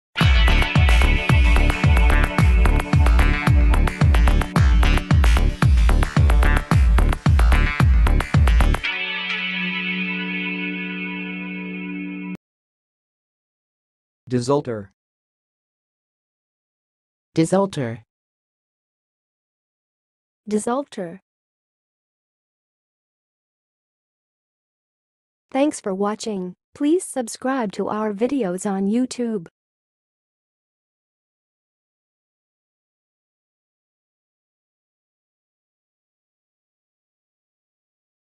How to say "desultor"! (High Quality Voices)
Watch in this video how to say and pronounce "desultor"!